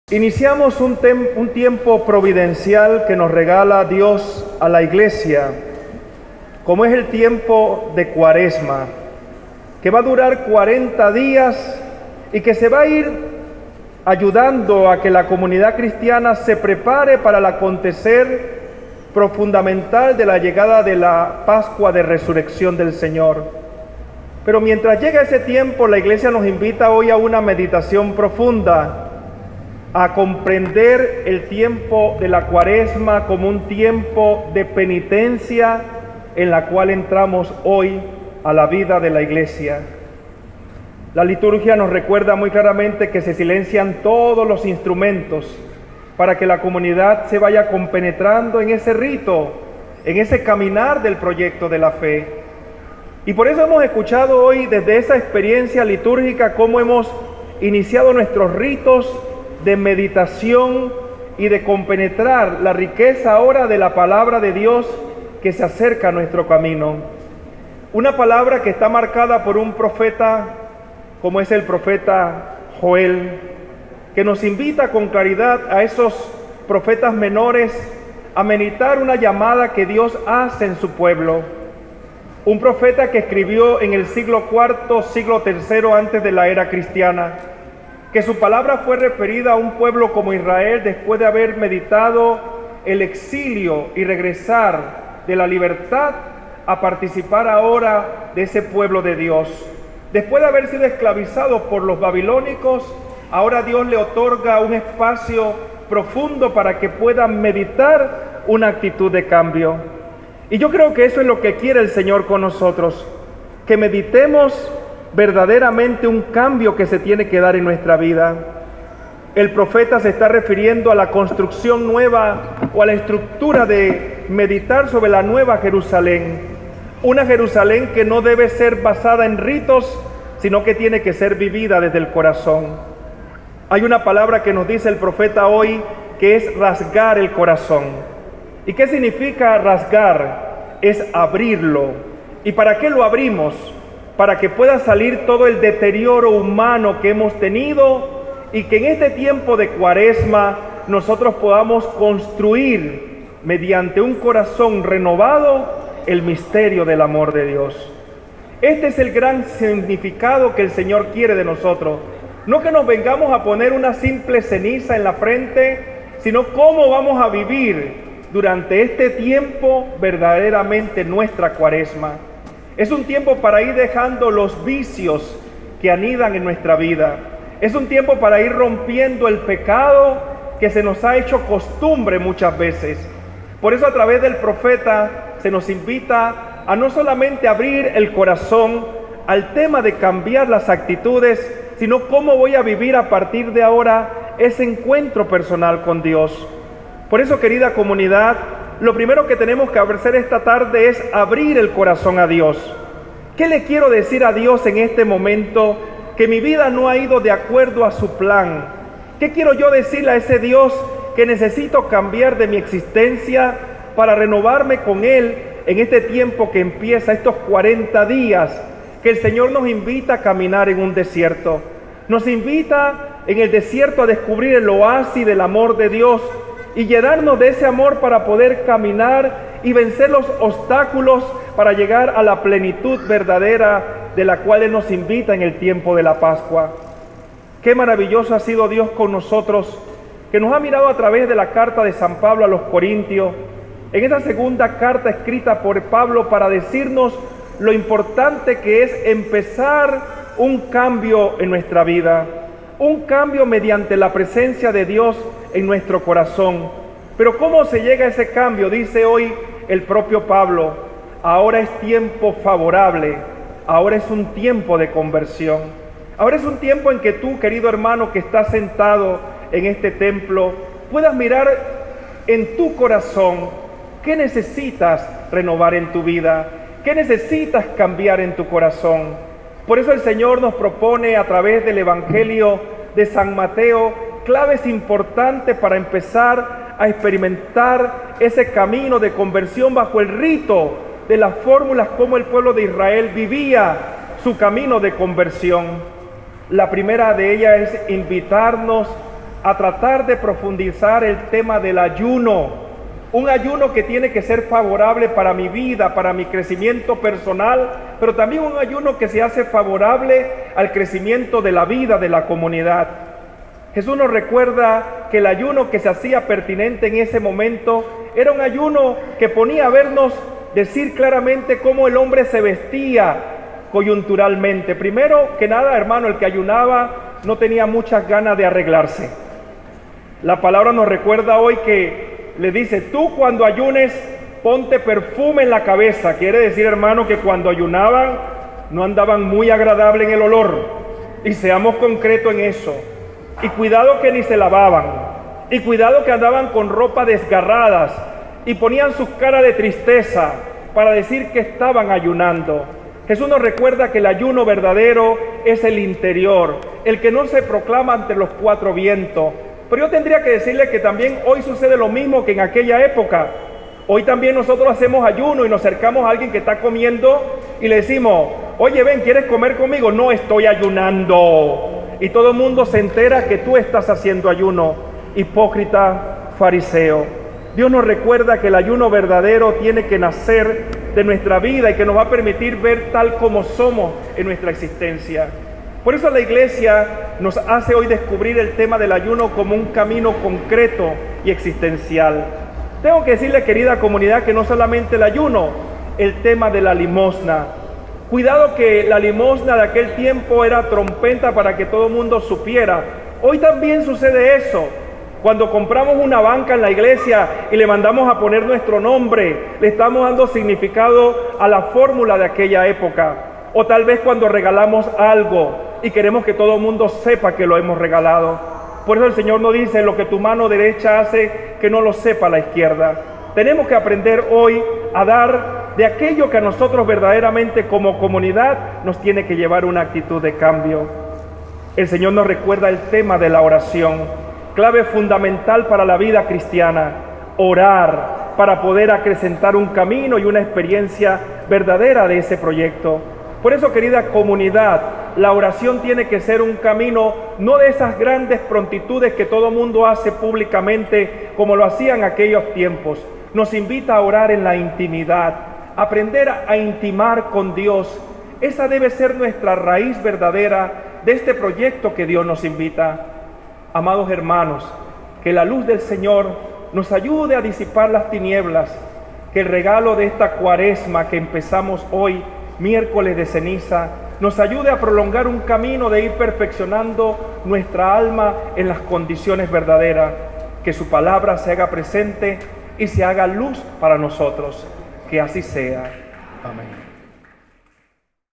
Homilía del Padre Jorge del Miércoles de Ceniza
Homilía de inicio de cuaresma desde la Parroquia Inmaculada Concepción de La Chorrera República de Panamà